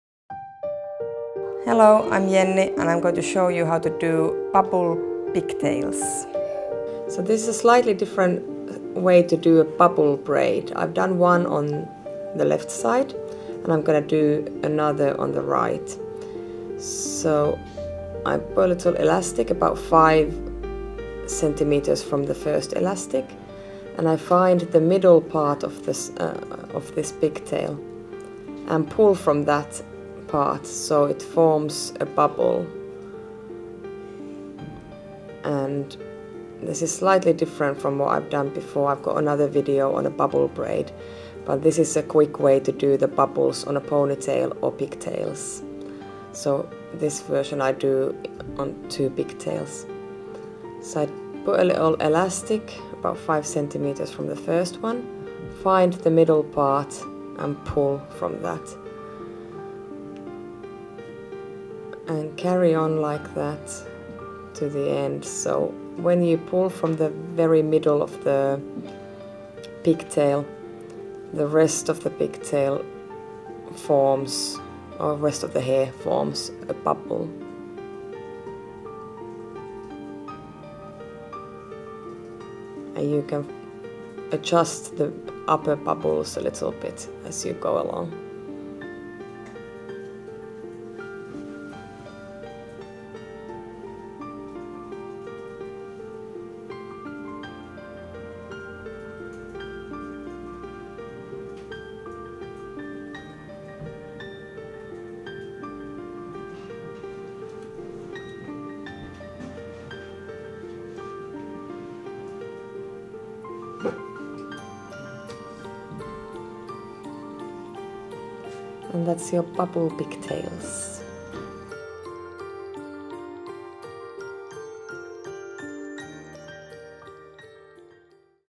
JHD bubble pigtails
This video shows a quick and easy way to do bubble pigtails.

You will need 10-16 hair elastics, try different colours for fun!

Film by: Veli Creative